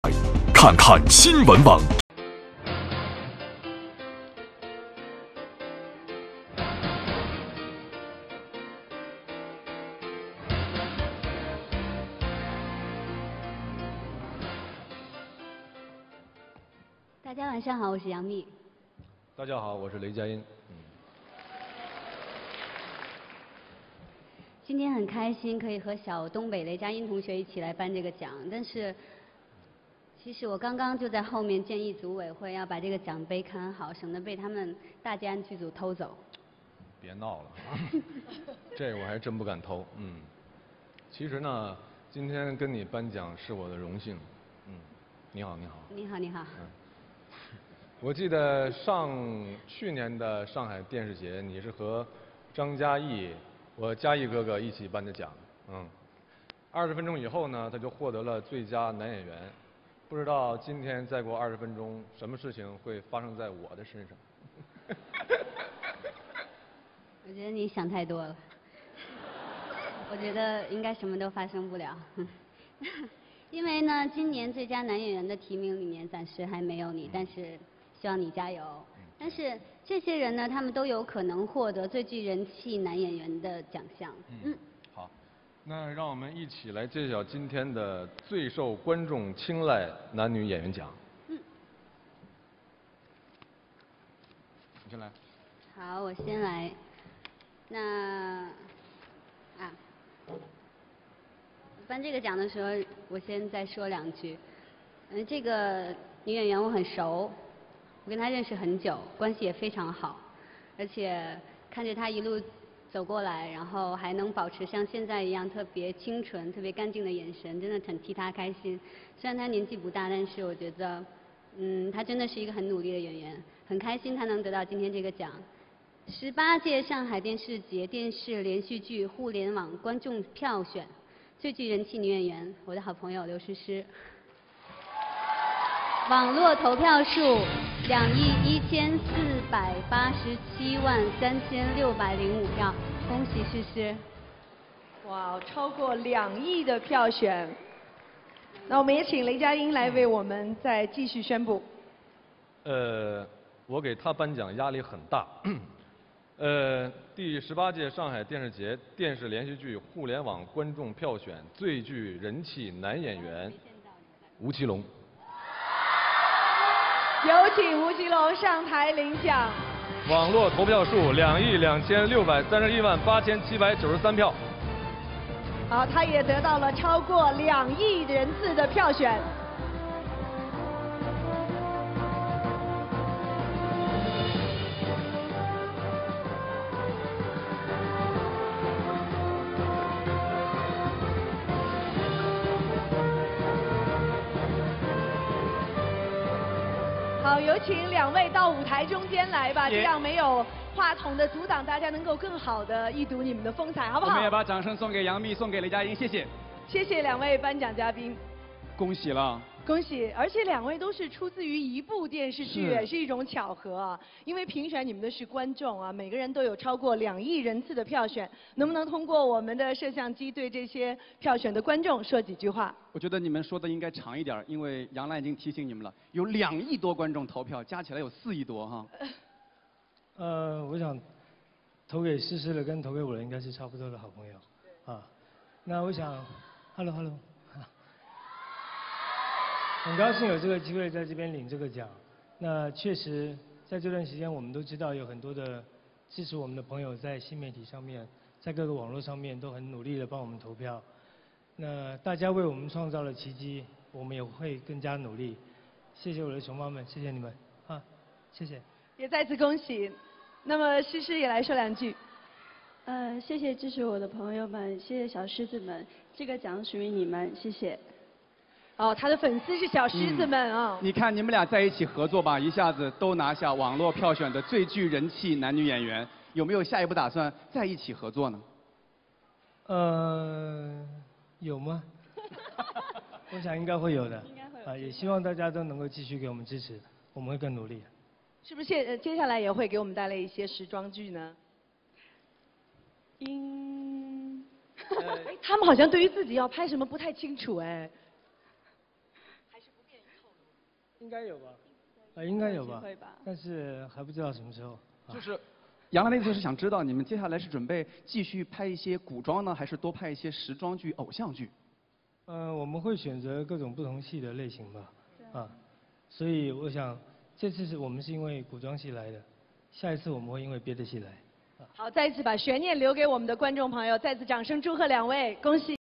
上海电视节：刘诗诗吴奇隆获最具人气男女演员奖.mp4
2012年6月15日，第18届上海电视节白玉兰颁奖典礼高清视频，雷佳音、杨幂颁发第18届上海电视节最具人气男女演员奖，获奖男女演员分别是刘诗诗、吴奇隆，他们分别在网络上得到超过两亿粉丝的支持。